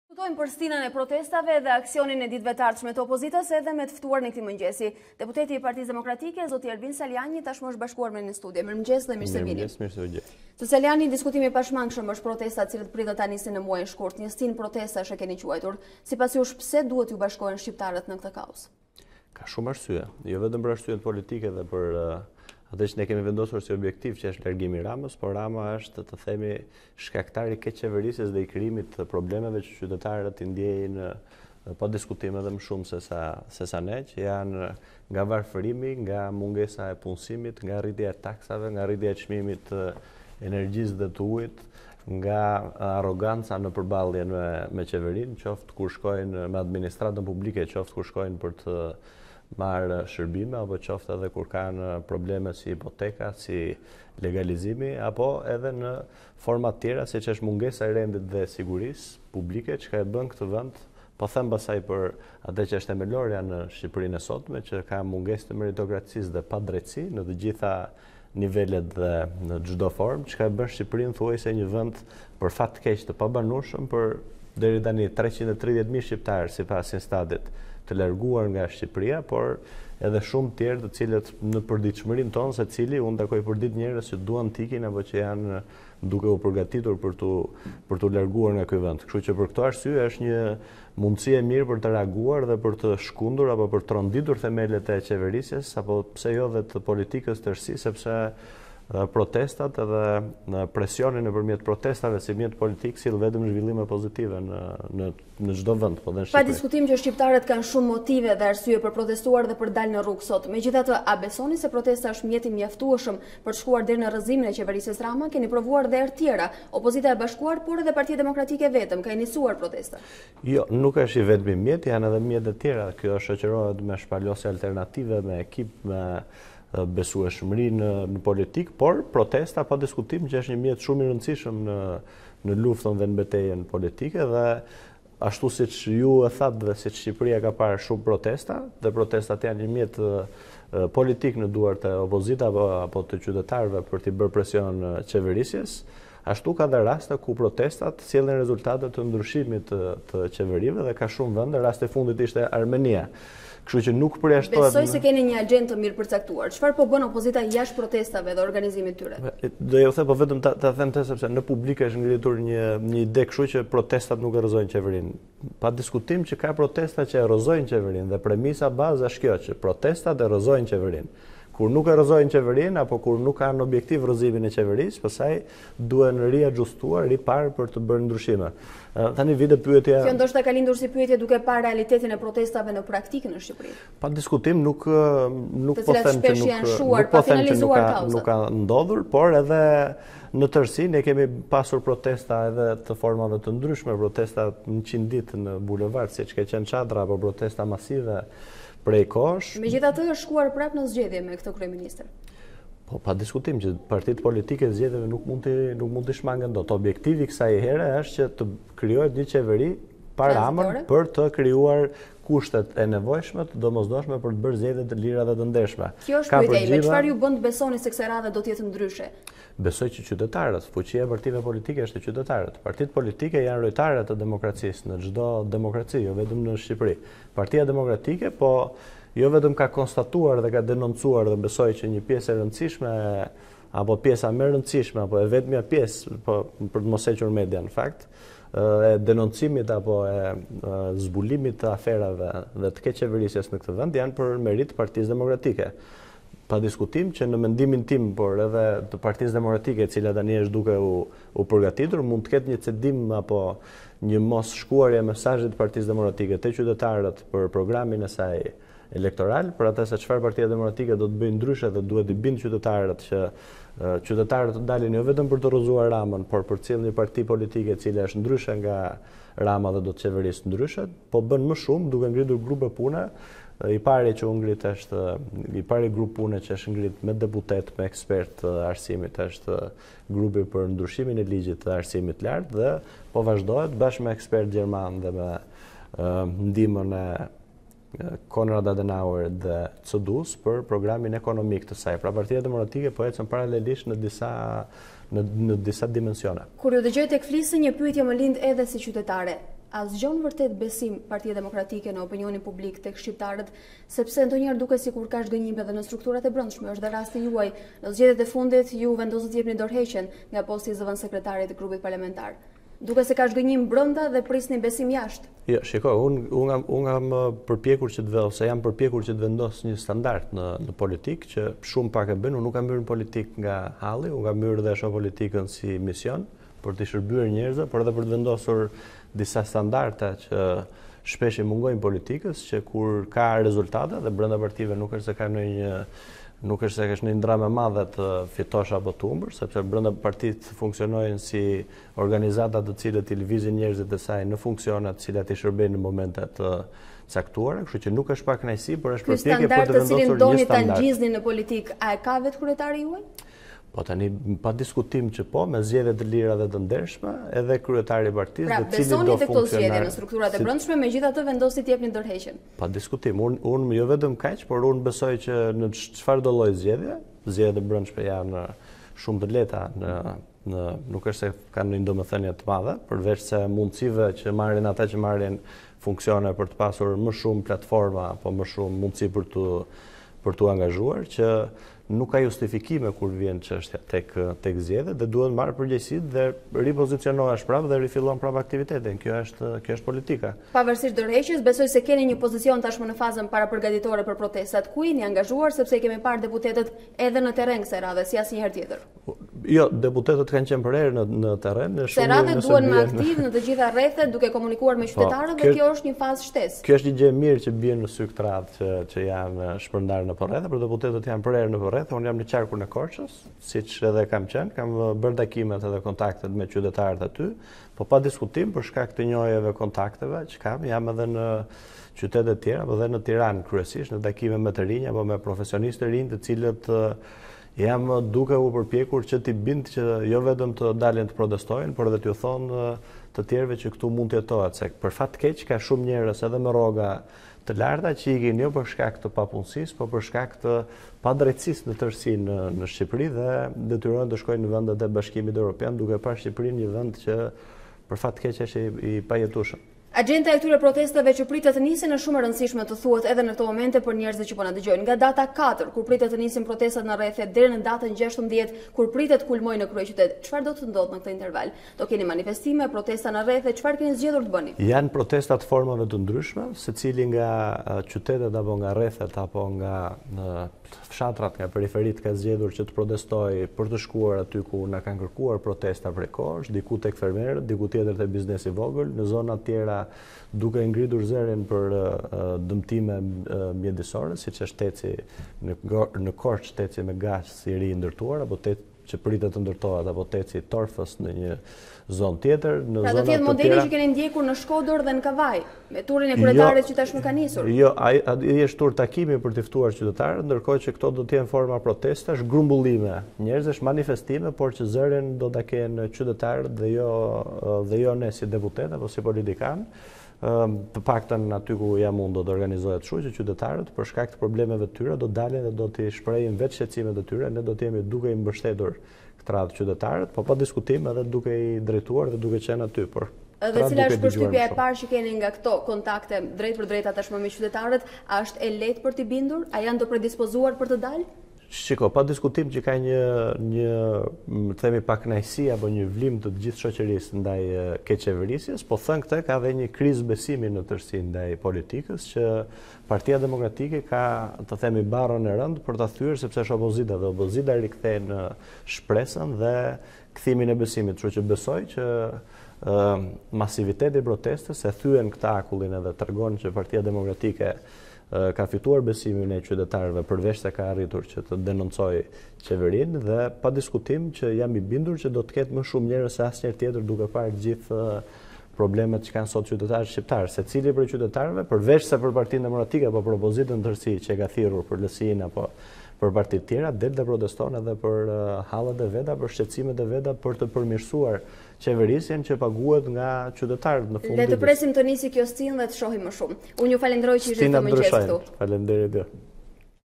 Salianji: Dorëheqja? Jam përpjekur të vendos një standard
Një nga figurat emergjente të Partisë Demokratike, Ervin Salianji ishte i ftuar në studiot e A2 për të folur për përgatitjet e protestës së madhe të opozitës më 16 shkurt.
Salianji, pasi ka nënvizuar që vendi është tashmë“ pothuajse i pabanueshëm për shqiptarët, jo vetëm për arsye ekonomike por në radhë të parë për “padrejtësitë” që jetojnë çdo ditë, pohoi se protesta do të jetë një test edhe për opozitën, që duhet të thyej “mosbesimin ndaj politikës” që karakterizon opinion publik.
Sipas tij, “ presioni ndërmjet protestave sjellin vetëm rezultate pozitive, dhe nuk është e vërtetë që protestat nuk rrëzojnë qeveritë. Rasti i fundit është në Armeni. Kur nuk kanë këtë objektiv, atëherë duhen ndryshuar”.
Ai pranon që mund të ketë pasur “një cedim në mos përcjelljen e mesazhit të elektoratit për atë që PD duhet të bëj ndryshe nga Rama” , por vlerëson se PD  është duke ngritur grupe pune, “si ai për ndryshimin e Arsimit të lartë, me ndihmën e Konrad Adenuar dhe CDU-së gjermane.
Në planin personal, Salianji vendosi të japë dorëheqjen nga sekretar i grupit. Për të kjo nuk është domethënëse, sepse “Jam përpjekur të vendos një standard, nuk kam hyre nga halli por me një mision. Nuk është drame e madhe të fitosh apo të humbësh brenda partisë, nuk ka pra pakënaqësi”.